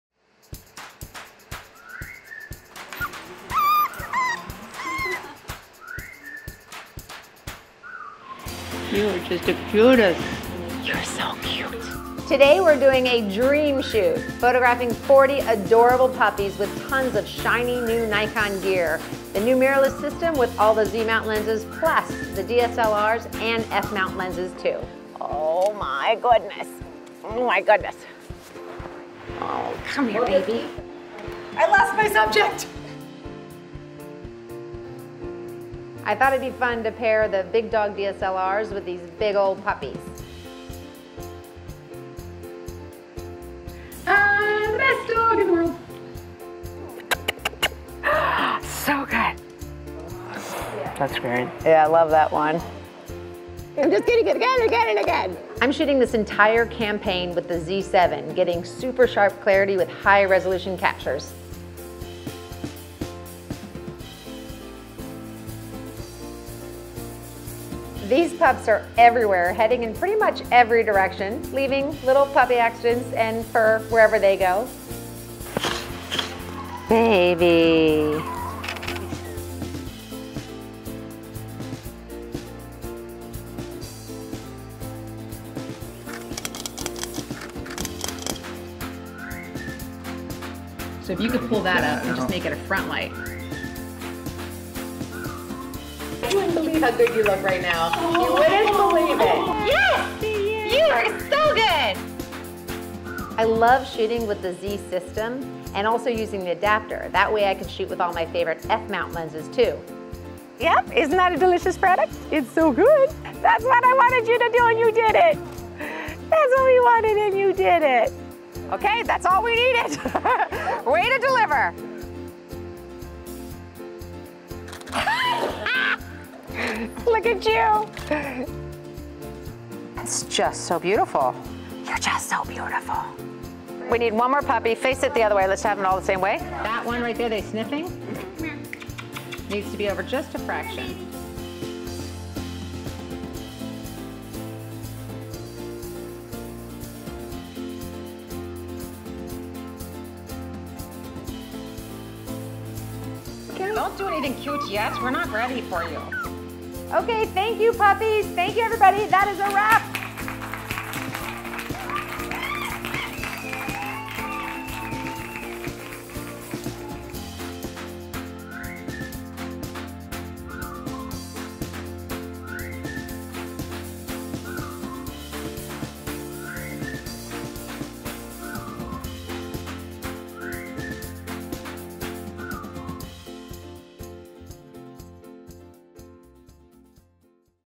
Puppy Portraits with Tamara Lackey
What’s better than photographing puppies? Photographing them alongside Nikon gear! Go behind-the-scenes to watch how Nikon Ambassador Tamara Lackey photographed these adorable puppies! Want to bring them home with you? Put a smile on your face every day by downloading these heart-melting wallpapers for your desktop and smart devices